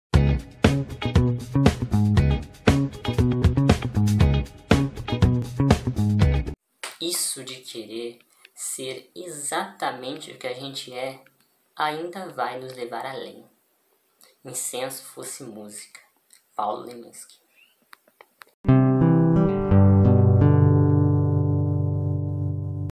Incenso fosse música - Zueira Literária (Tomo 86)
Hoje, na Zueira Literária, a poesia de Paulo Lemiski por Matheus Guilherme Antunes.

Deleitem-se.

Concepção: Ribamar Jr
Edição: Jarod Mateus

LABORATÓRIO DE LEITURA E PRODUÇÃO TEXTUAL
Coordenação: Prof. Ribamar Jr

TV RADIOTEC
Você conectad@ ao conhecimento